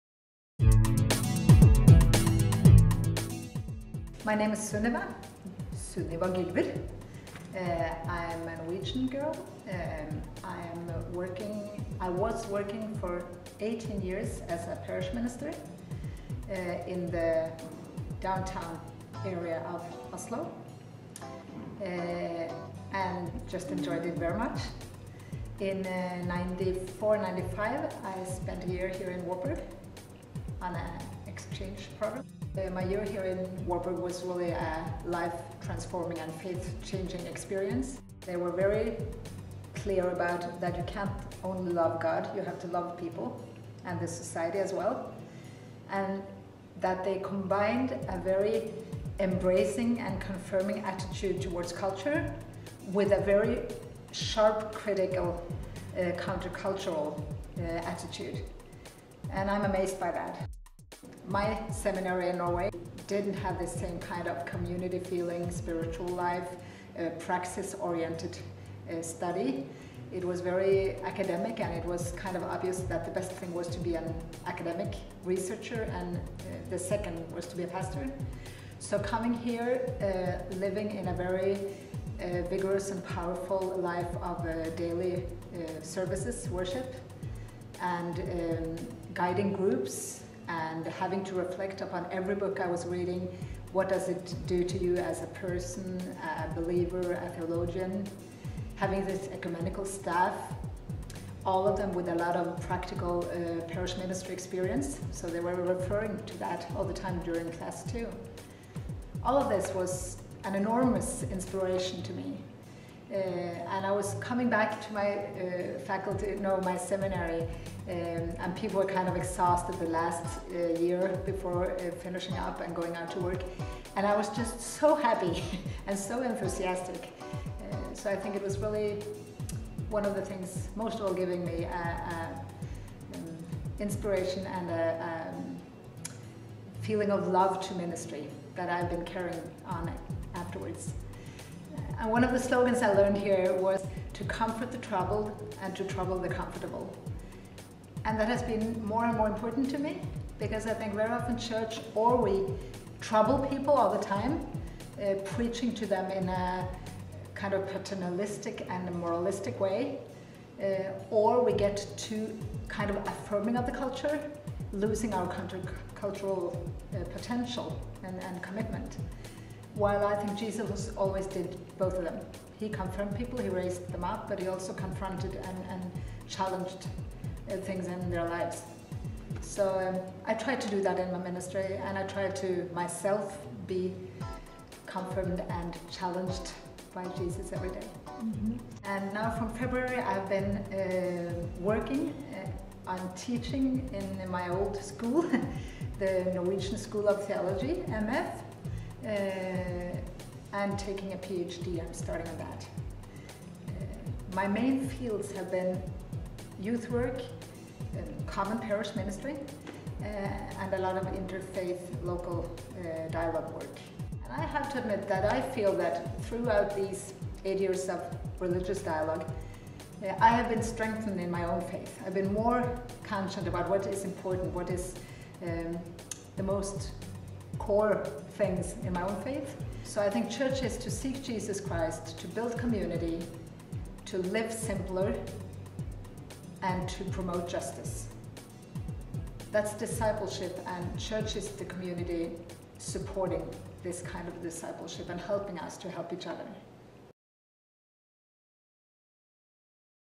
Sunniva Gylver
Sunniva Gylver is a pastor in Norway who studied abroad at Wartburg Theological Seminary from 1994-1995.  In this video she talks about why she chose to study at Wartburg and how it impacted her ministry.